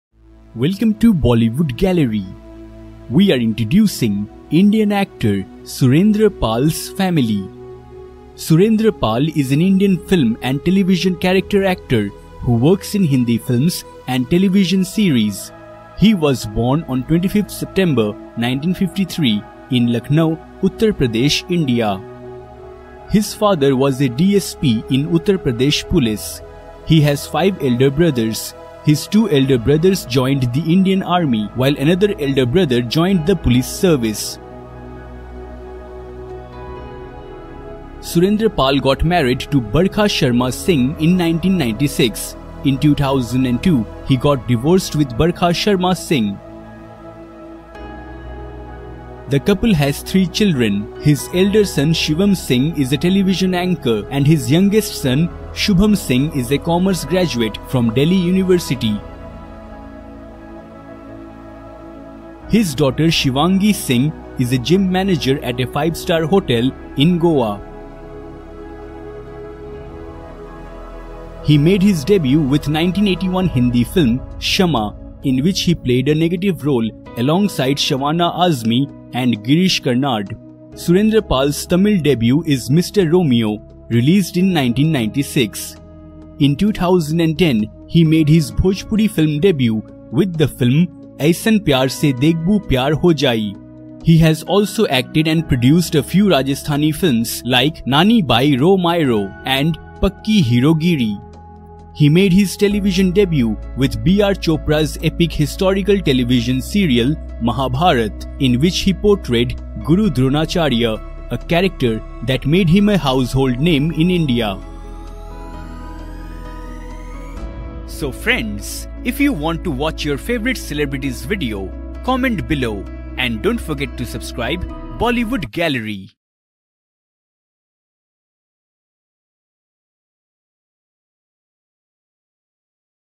Surendra Pal Family With Parents, Wife, Son, Daughter, Career & Biography
Surendra Pal was born on 25 September 1953 in Lucknow, Uttar Pradesh, India. His father was a DSP in Uttar Pradesh Police. He has five elder brothers. His two elder brothers joined the Indian Army & another elder brother joined the police service.

Surendra Pal got married to Barkha Sharma Singh in 1996. In 2002, he got divorced with Barkha Sharma Singh. The couple has three children. His elder son, Shivam Singh is a television anchor and his youngest son, Shubham Singh is a Commerce graduate from Delhi University. His daughter, Shivangi Singh is a Gym Manager at a five-star hotel in Goa.